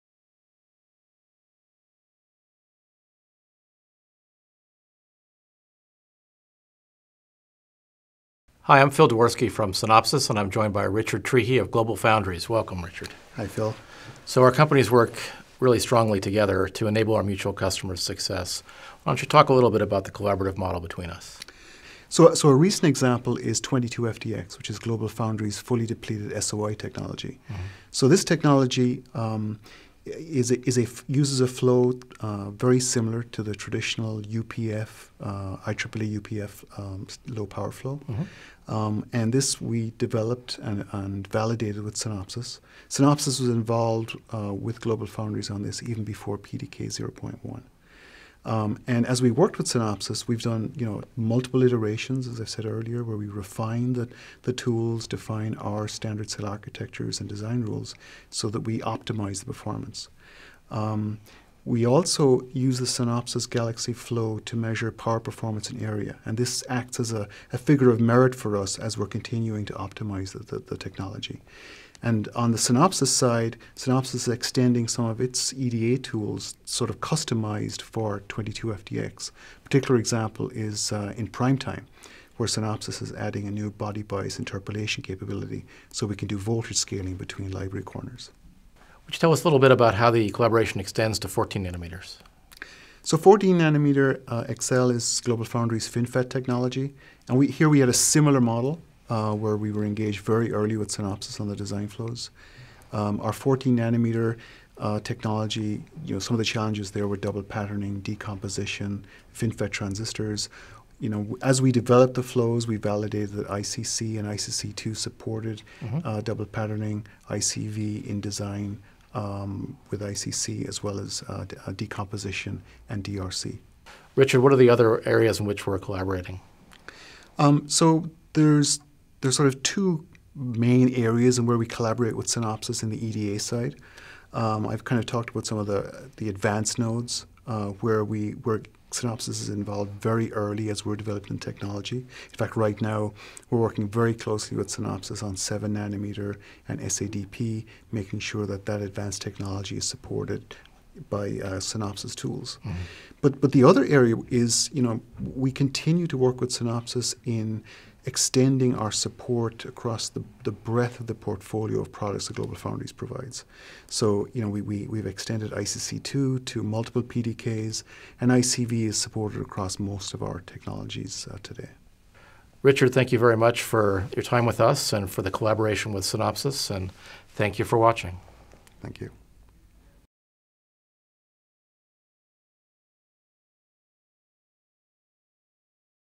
GLOBALFOUNDRIES & Synopsys: Collaborating to Enable Advanced Design
Richard Trihy, Director, Design Methodology at GLOBALFOUNDRIES, discusses the ways in which ARM and Synopsys are collaborating to achieve the world’s most advanced designs.